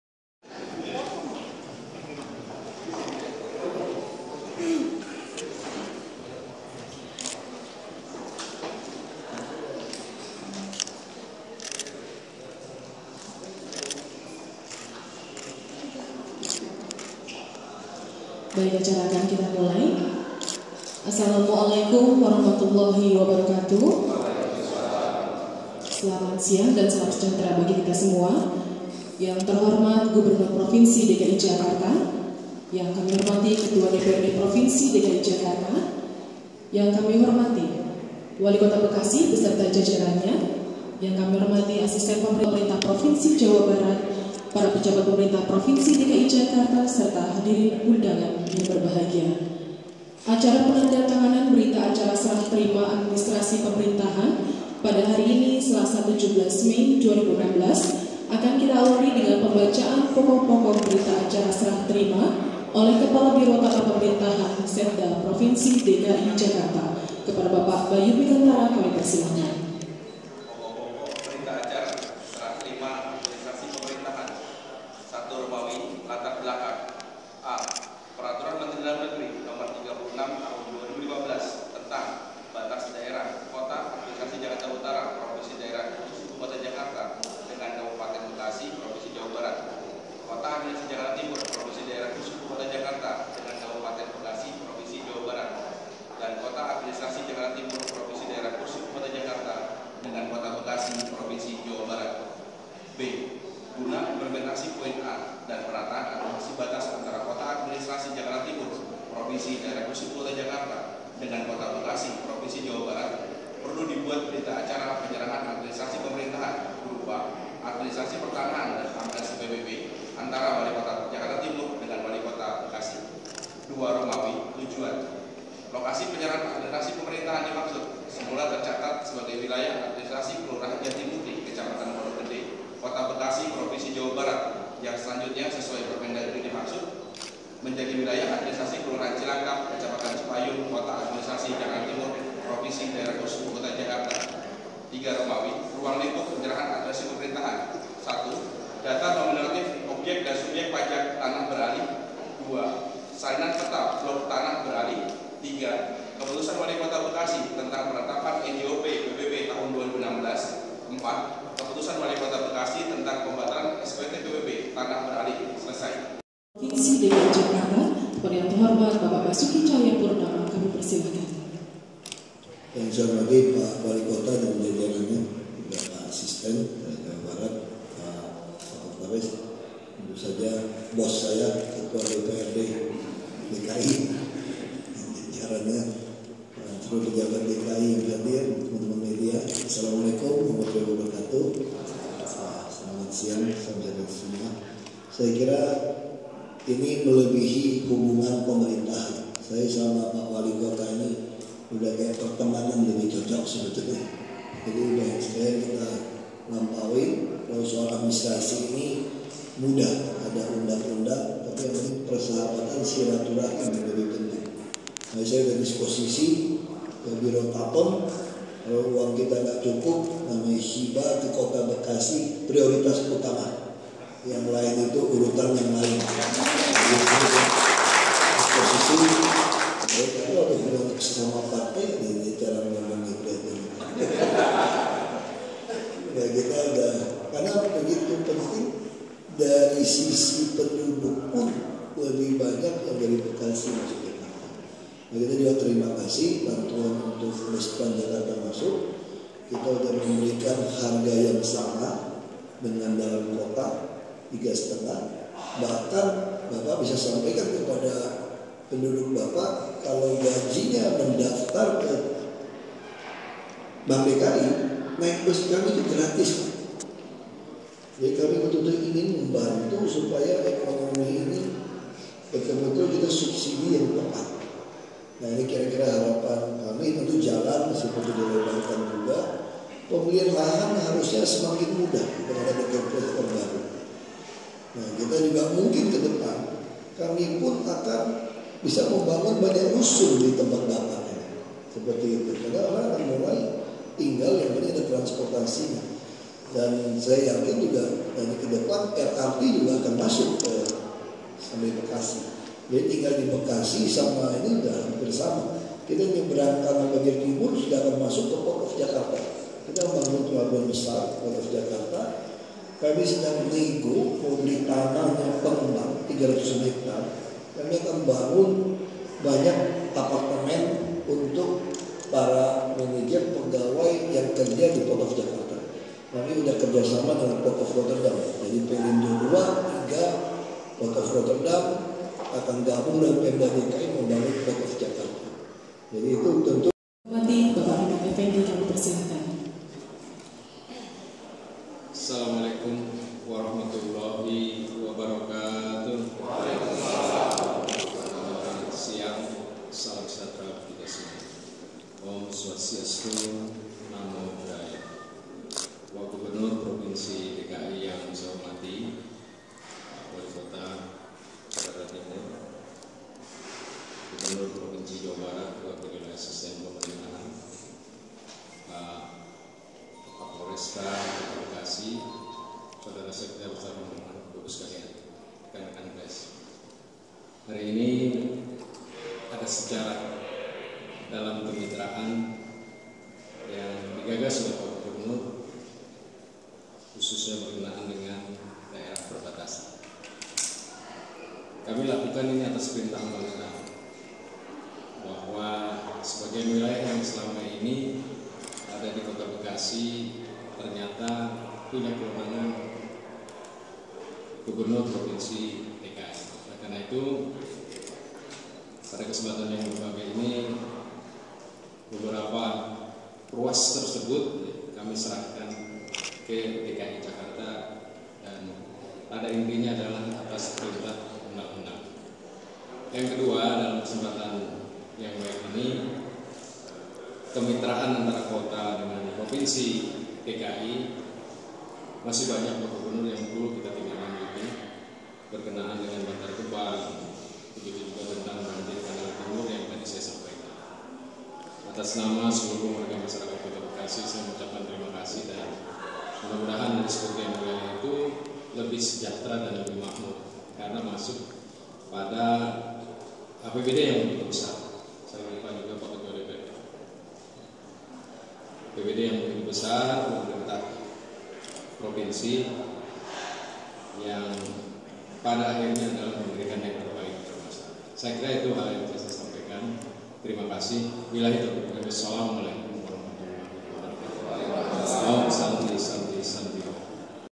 Penandatangan BA Serah Terima Administrasi Pemerintahan Antara Pemprov. DKI dengan Pemkot Bekasi
Balai Kota Jakarta ( 17/5) - Gubernur Daerah Khusus Ibukota (DKI) Jakarta Basuki Tjahja Purnama siang ini di Balai Kota Pemprov DKI menyaksikan penandatangan Berita Acara Administrasi Pemerintahan  antara Pemprov DKI dengan Pemkot Bekasi.

Pemerintah  Provinsi DKI Jakarta dan Pemkot Bekasi Kota Bekasi sebelumnya telah menyepakati penyerahan lahan seluas 5,1 hektare di lokasi perbatasan antara Kelurahan Jatiwarna, Kecamatan Pondokmelati dengan Kecamatan Cipayung, Jakarta Timur sesuai Peraturan Menteri Dalam Negeri Nomor 36 Tentang Batas Daerah Kota Administrasi Jakarta Utara Provinsi Daerah Khusus Ibukota Jakarta Dengan Kabupaten Bekasi Provinsi Jawa Barat, Kota Administrasi Jakarta Timur Provinsi Daerah Khusus Ibukota Jakarta Dengan Kabupaten Bekasi Provinsi Jawa Barat Dan Kota Administrasi Jakarta Timur Provinsi Daerah Khusus Ibukota Jakarta Dengan Kota Bekasi Provinsi Jawa Barat , yang rencananya akan dijadikan kolam retensi penanggulangan banjir.